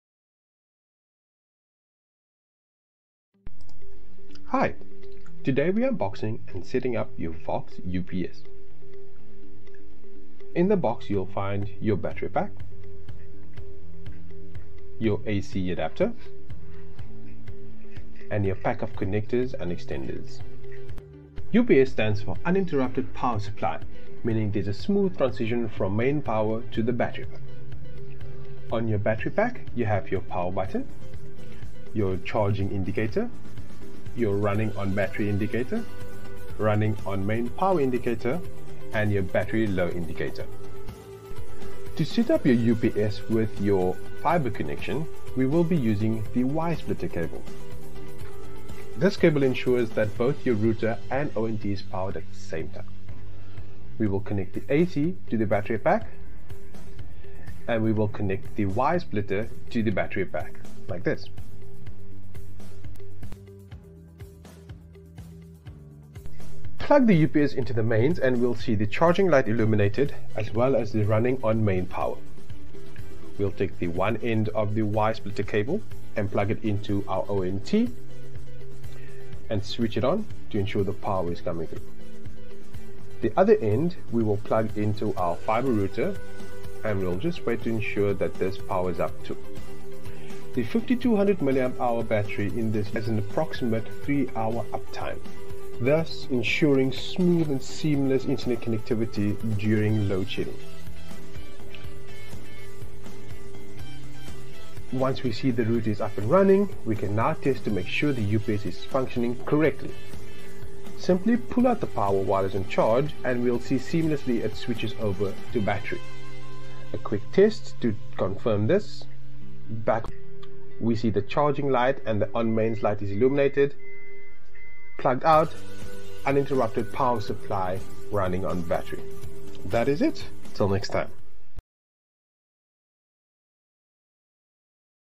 [ HOW TO ] set up your Vox UPS for a Fibre router
Watch this video to find out how to easily set up your Vox UPS to a Fibre router! 🔋💡 Get more done during power cuts & load shedding with an Uninterrupted Power Supply.